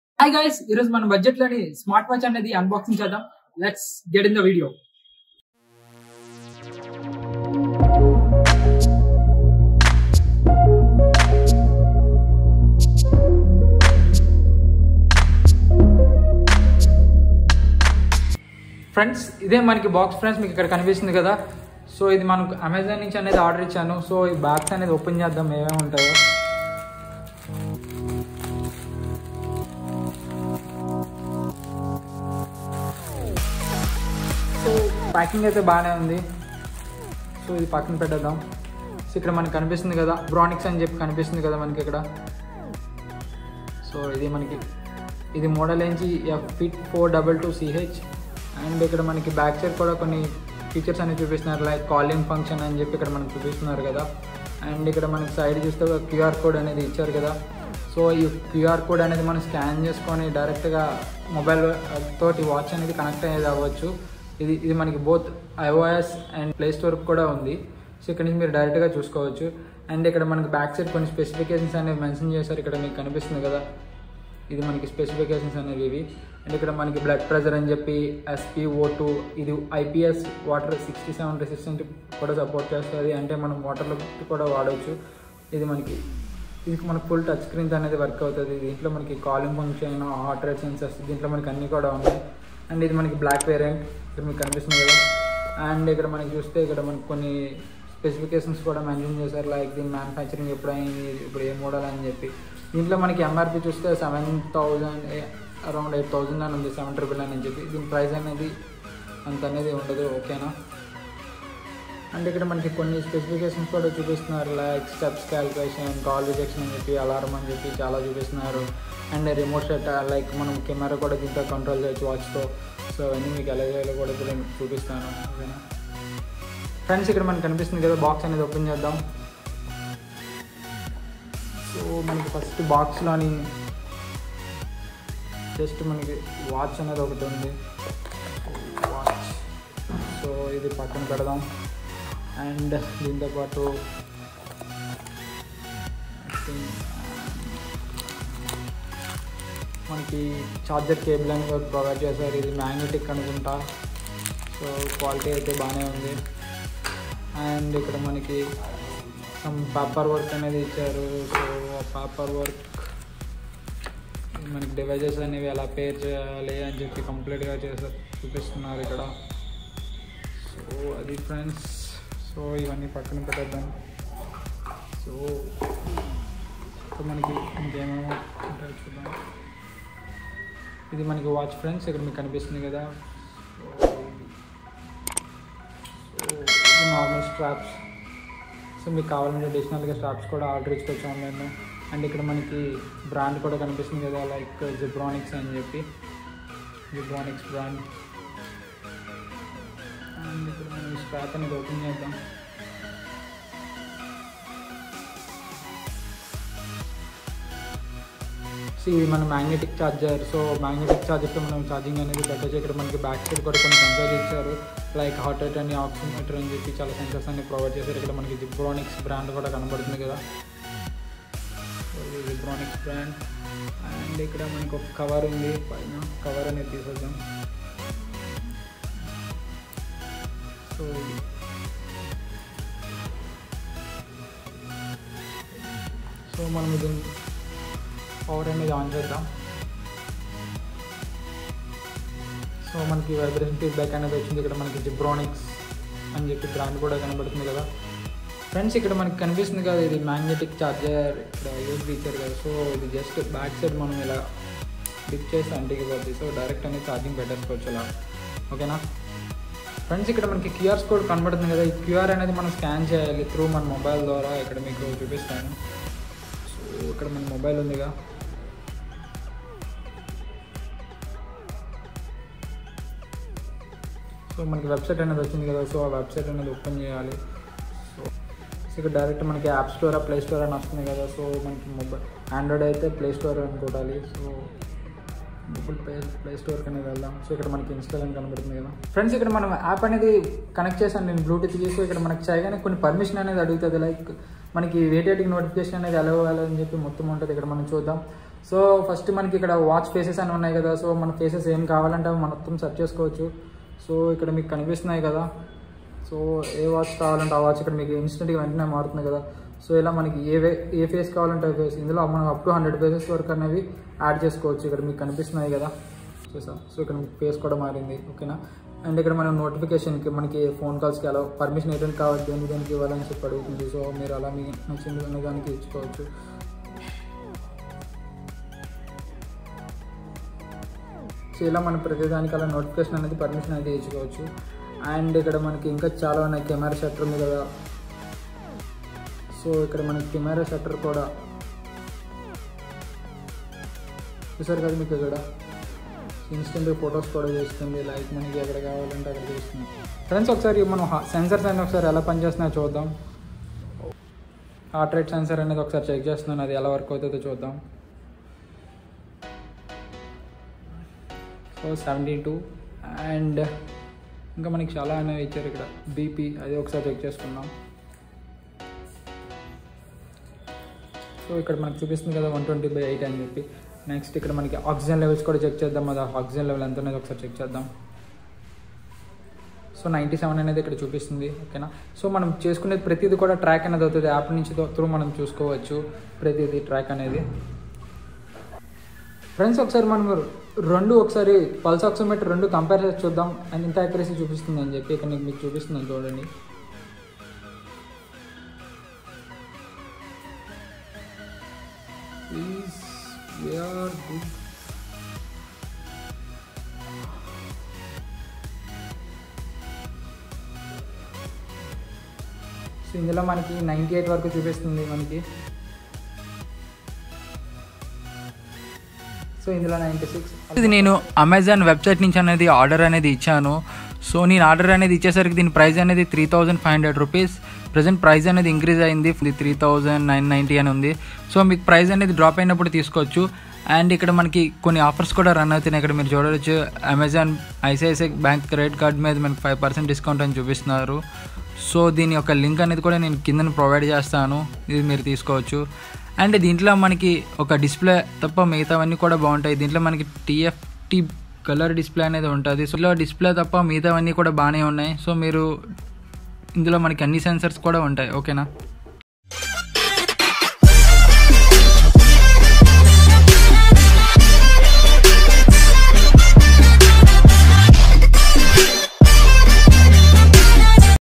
Zebronics Smart Watch Calling Feature Unboxing & Review ll in Telugu ll
Following Links

Twitter zebronics smart watch zebronics zeb fit 4220ch smart watch zebronics zeb fit 4220ch review zebronics fit 4220ch